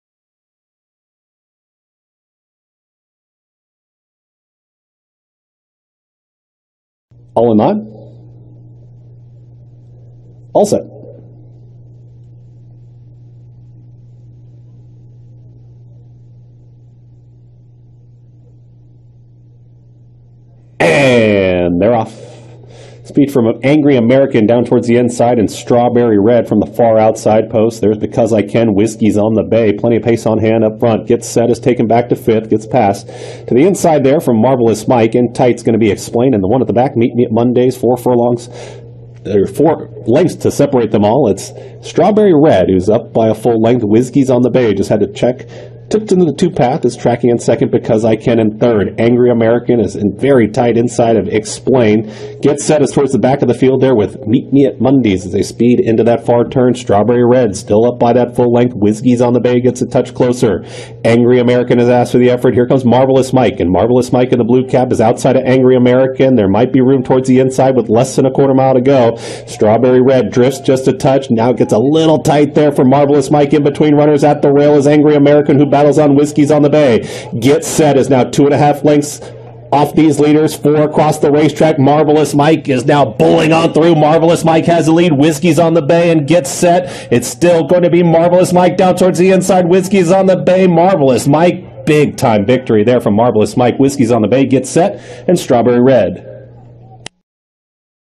Wizky's On The Bay PARX June 9
Justin Harguth and UTCB Stud's Wizky's On The Bay finished 2nd in a PA Bred Allowance going 6 furlongs with Silvestre Gonzalez at PARX on June 9, 2021